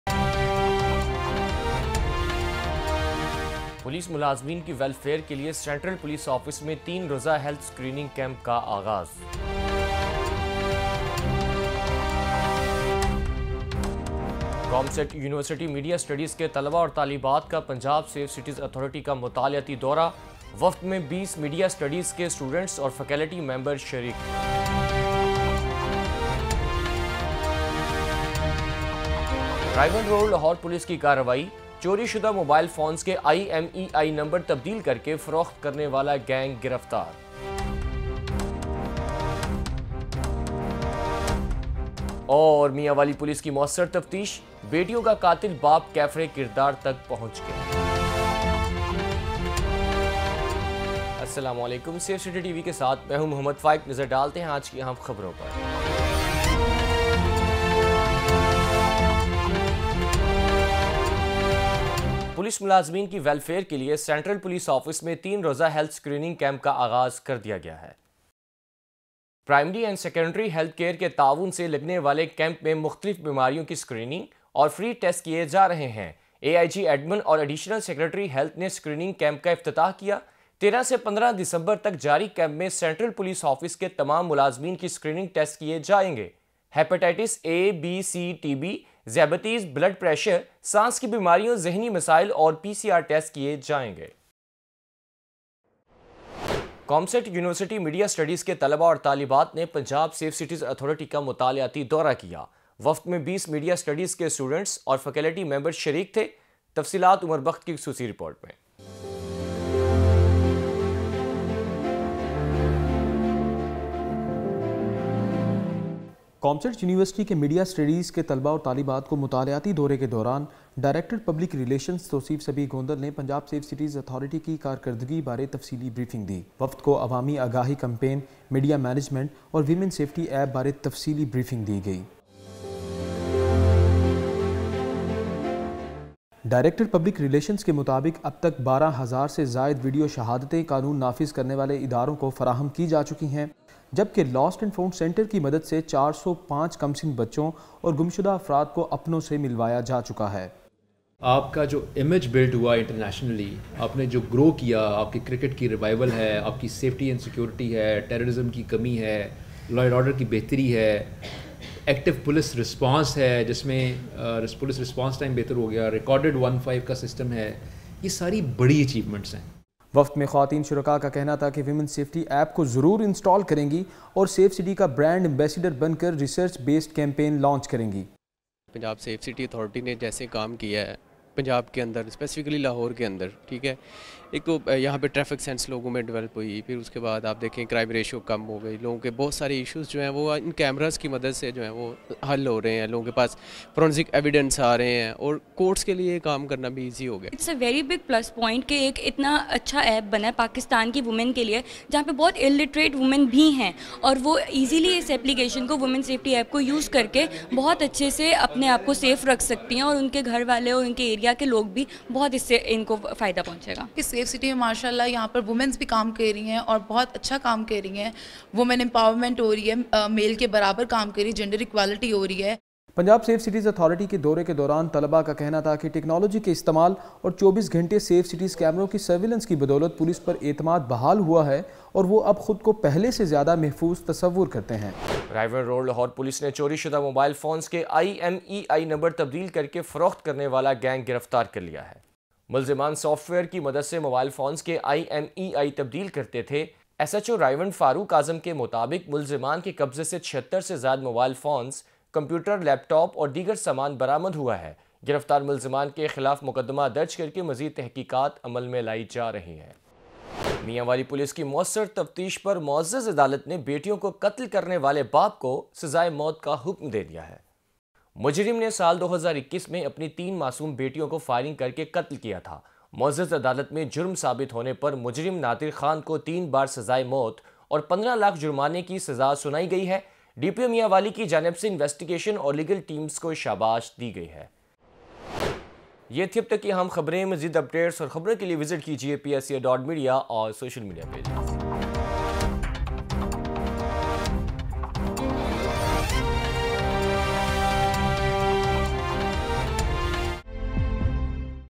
Three-day health screening camp started at Central Police Office for the welfare of police employees
Three-day health screening camp started at the Central Police Office for the welfare of police employees.
Students of Comsats University Media Studies visited Punjab Safe Cities Authority.
Gang arrested for selling stolen mobile phones by changing IMEI numbers.
Details are in bulletin